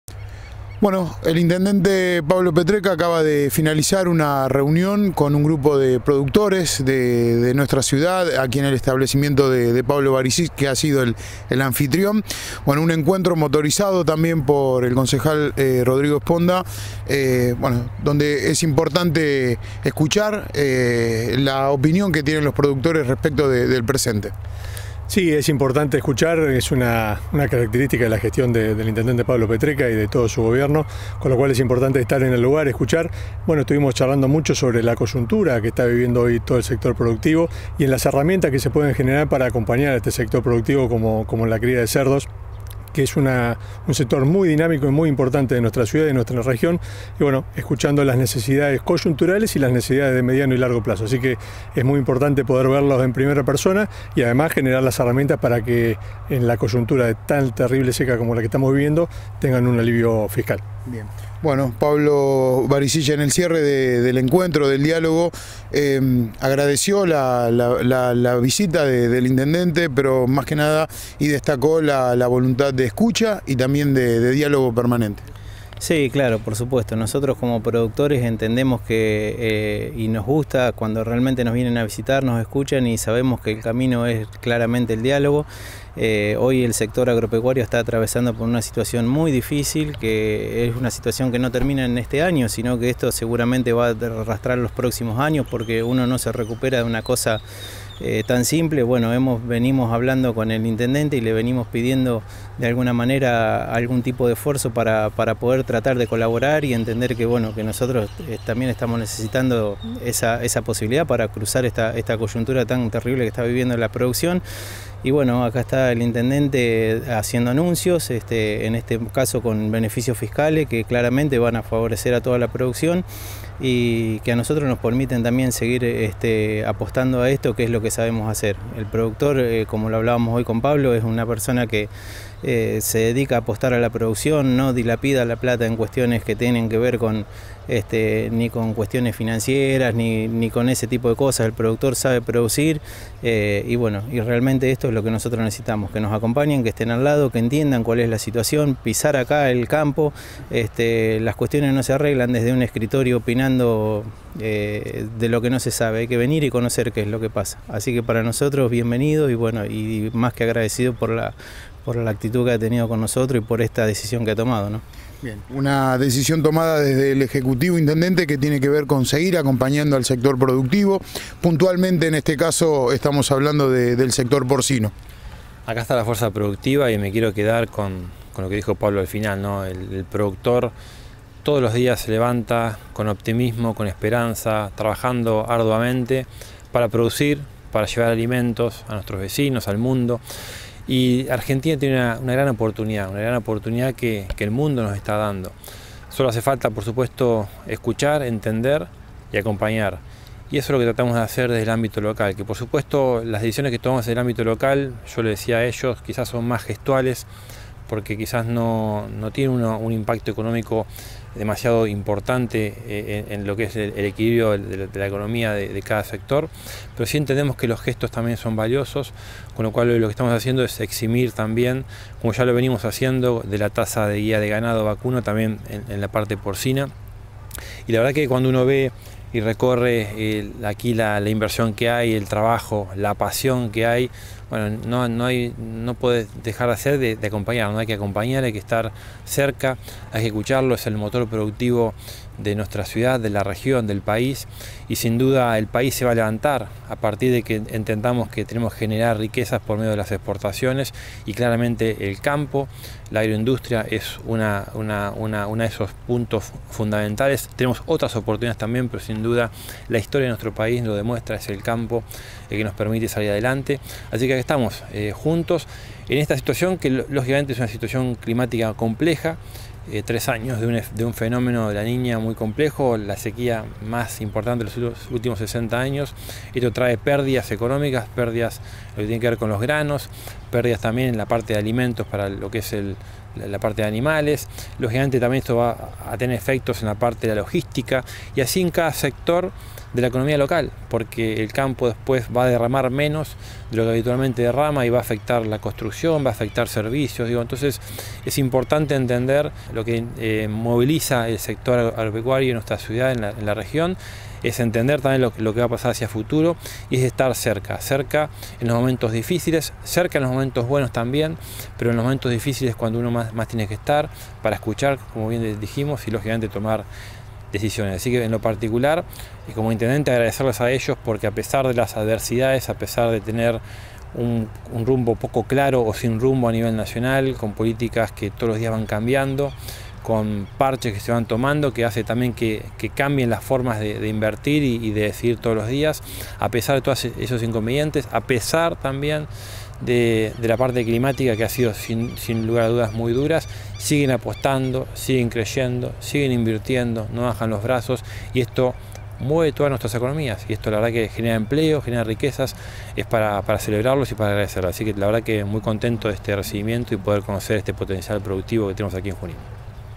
Petrecca anunció alivios fiscales para acompañar al sector productivo porcino
El intendente juninense Pablo Petrecca mantuvo una reunión con productores porcinos del Partido, con el propósito de conocer con mayor profundidad la compleja situación que atraviesan como consecuencia de la extrema sequía que afectó al sector y la falta de políticas claras por parte del Gobierno Nacional. En dicho encuentro, les informó que los eximirá del pago de la tasa de guía de traslado de ganado porcino. “Sabemos que son medidas que no tienen un gran impacto, pero creemos en la importancia de los gestos y en los momentos difíciles es cuando más hay que estar junto al campo”, afirmó el alcalde.